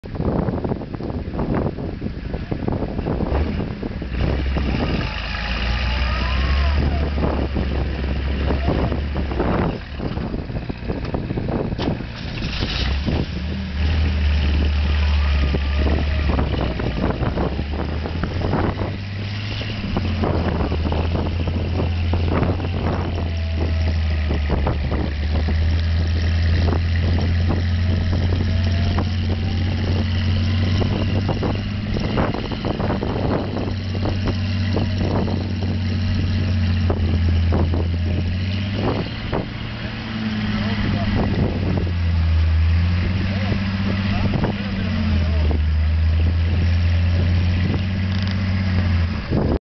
Muddy offroad
Anders stuck in the mud